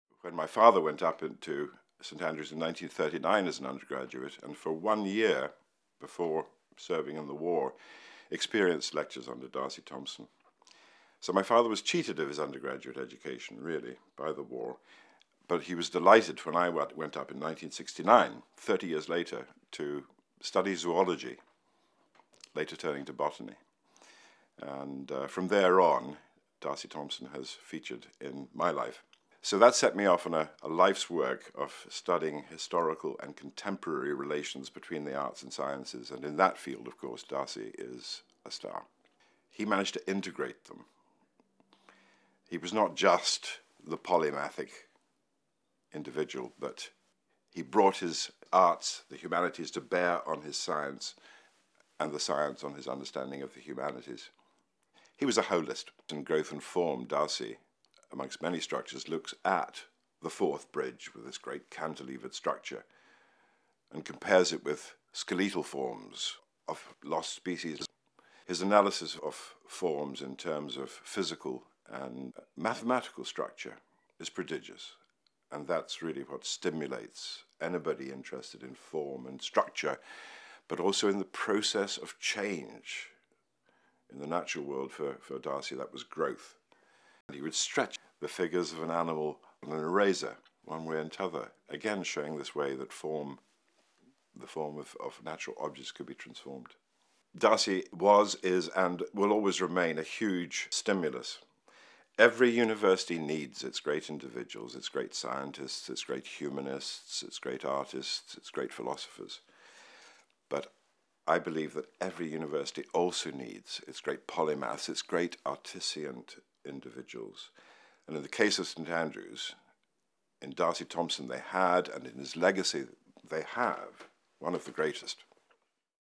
Colin Sanderson on D'Arcy Wentworth Thompson and Polymaths
A personal account primarily for family & friends, but ending with a strong statement about celebrating great polymaths.